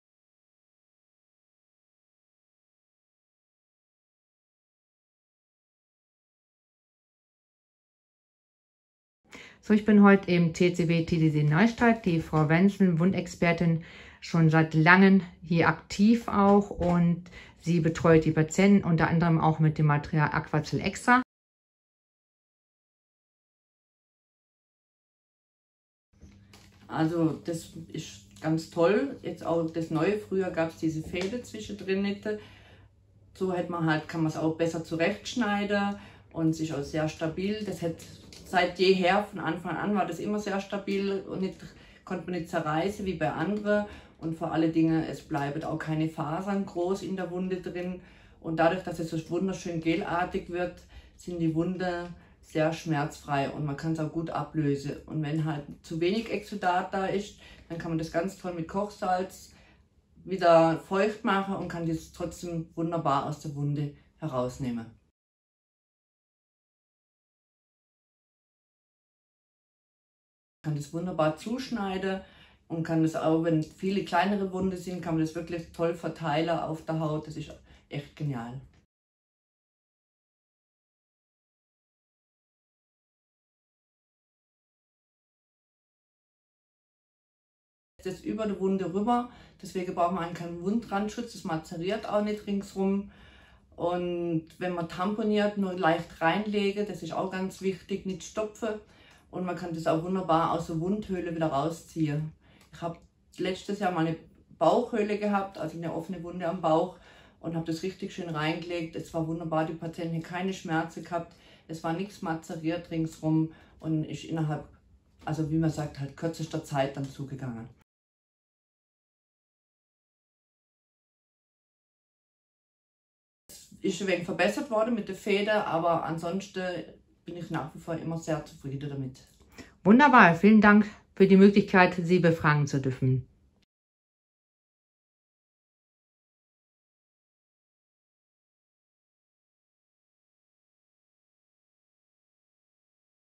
Interview: 25 Jahre AQUACEL® Wundverbände | Erfahrungen & Tipps
AQUACEL® feiert dieses Jahr 25. Geburtstag.
In dieser Zeit haben wir viele Veränderungen, Innovationen und Herausforderungen gesehen. Damals wie heute stehen wir an der Seite der Anwender und Patienten und unterstützen Sie mit Wundauflagen höchster Qualität, um die Wundversorgung zu verbessern.